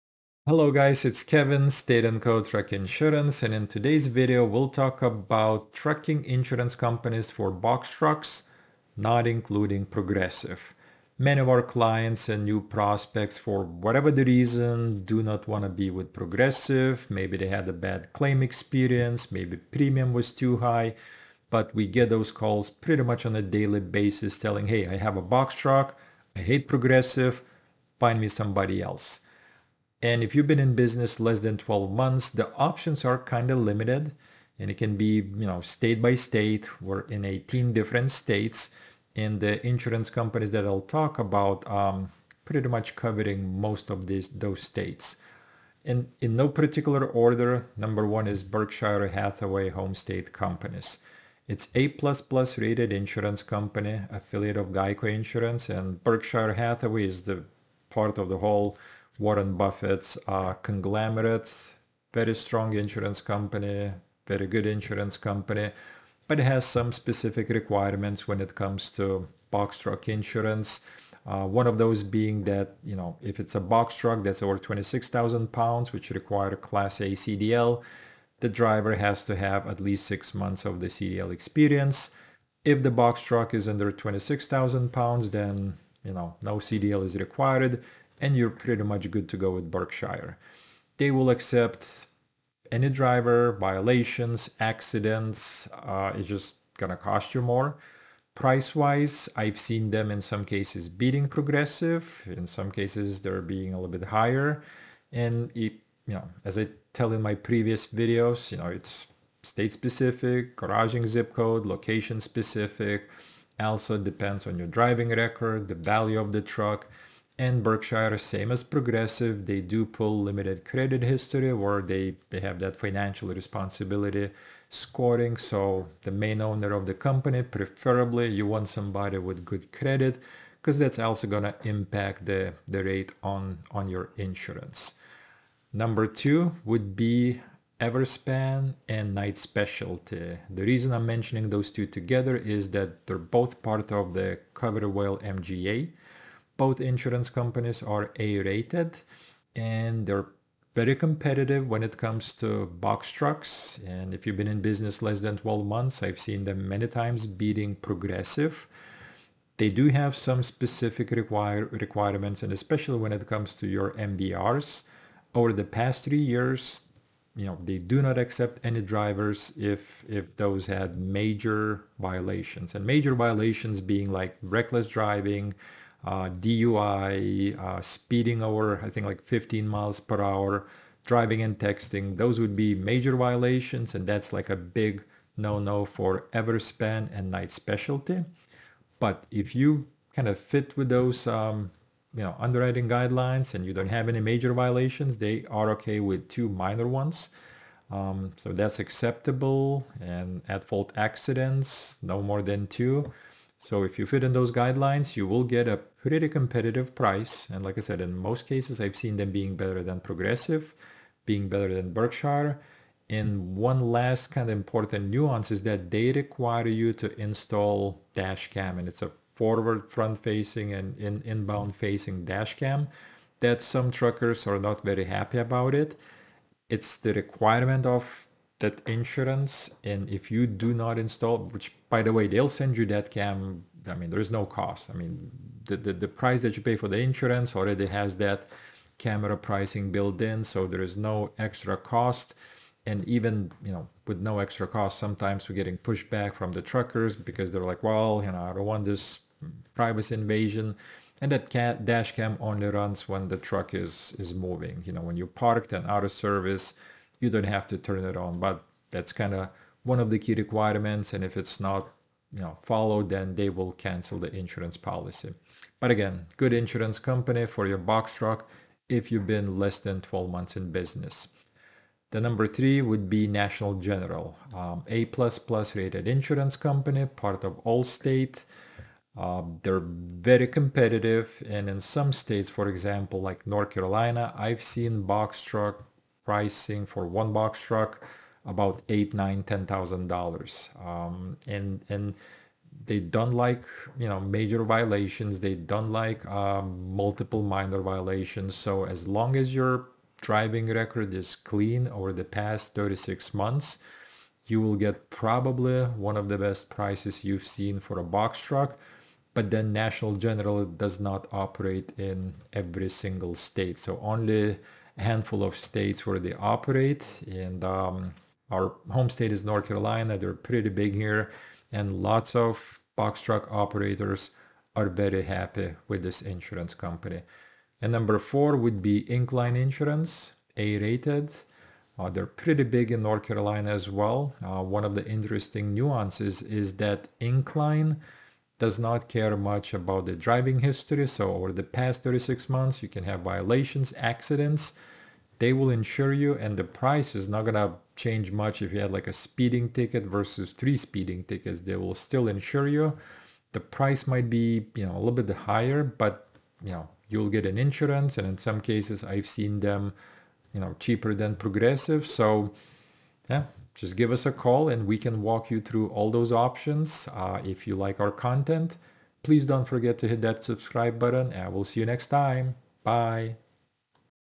Top Four Box Truck 🛻 Insurance Companies - Not Progressive
Other box truck insurance companies besides Progressive.